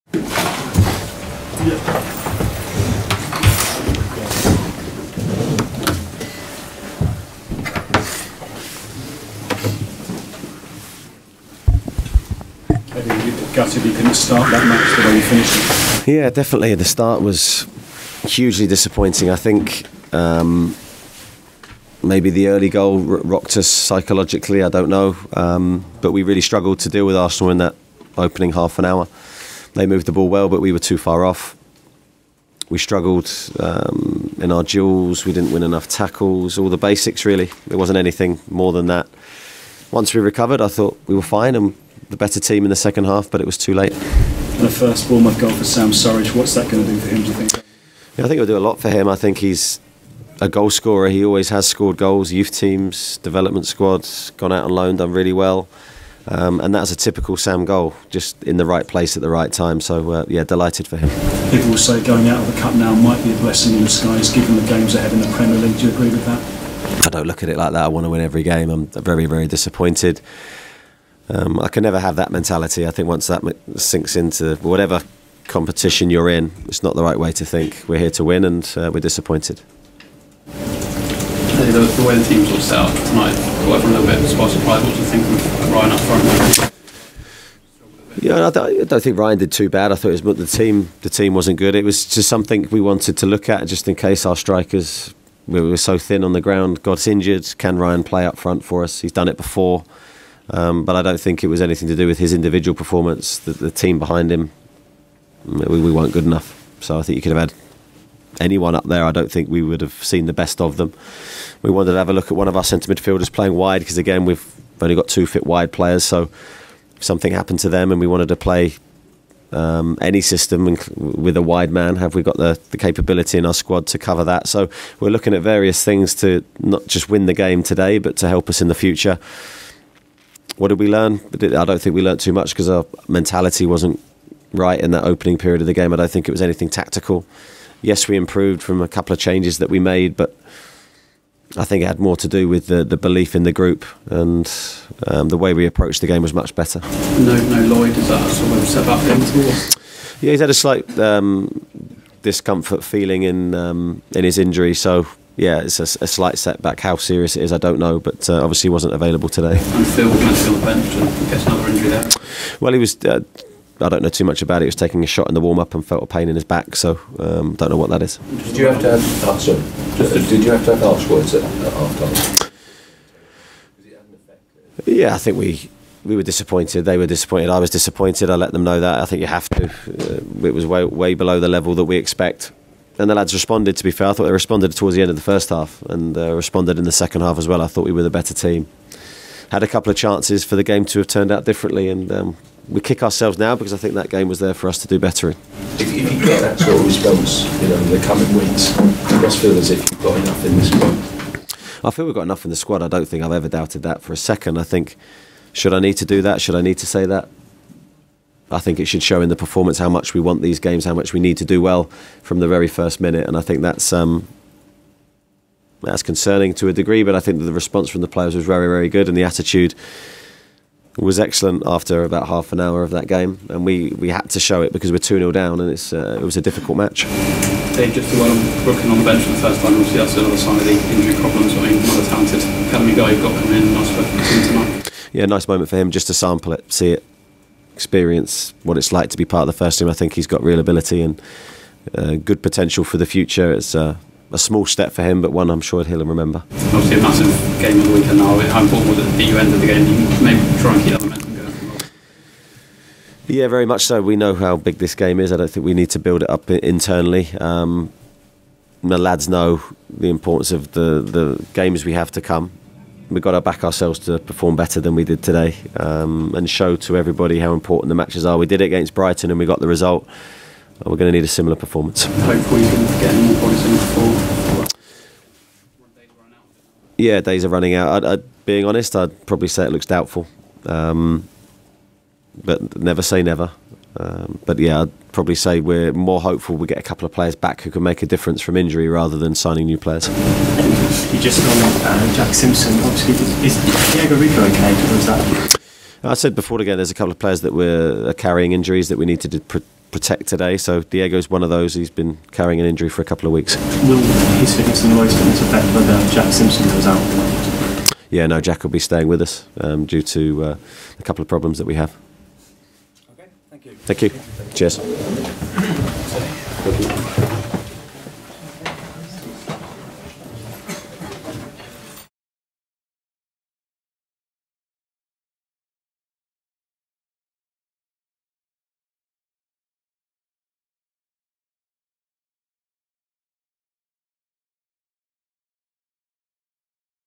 Bournemouth 1-2 Arsenal - Eddie Howe FULL Post Match Press Conference - FA Cup
Bournemouth manager Eddie Howe holds a post-match press conference after his team lost 2-1 at home to Arsenal in the fourth round of the FA Cup.